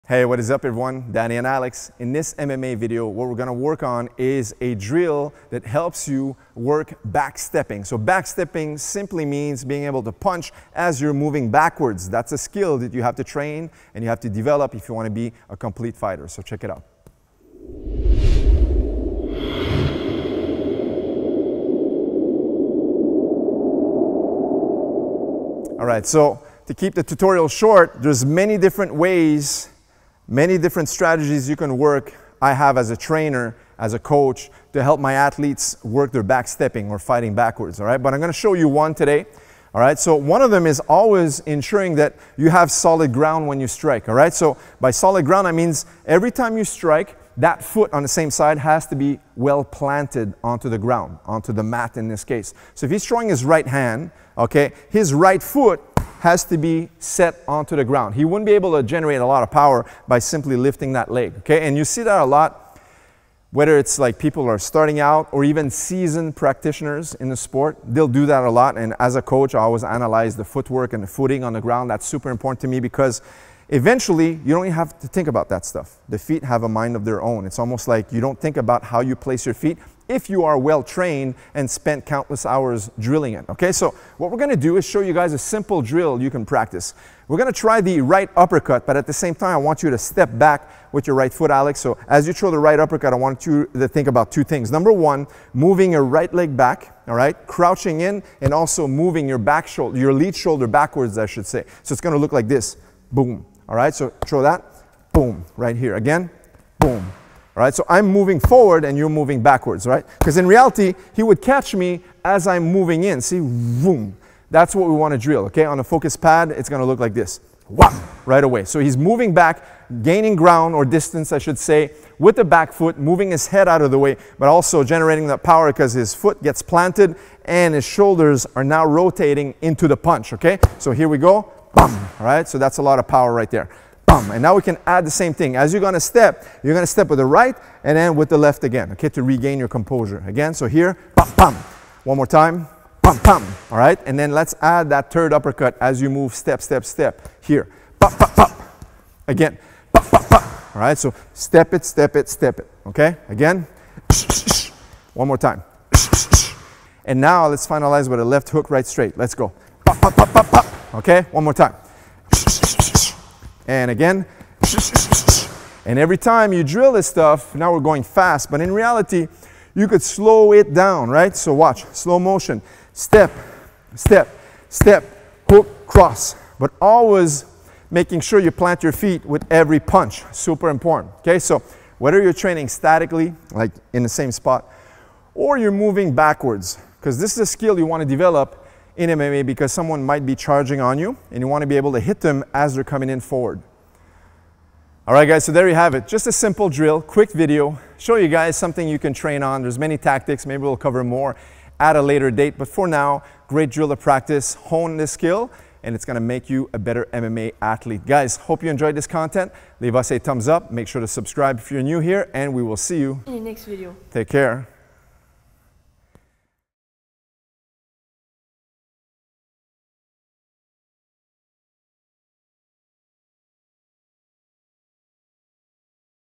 MMA | Backstepping with Your Punches
In this video, we show you how to punch (and generate power) while backing out. Backstepping, as we call it, is an important skill that should be practiced diligently in order to become a high-level striking specialist.
D&A.

KIDS & ADULTS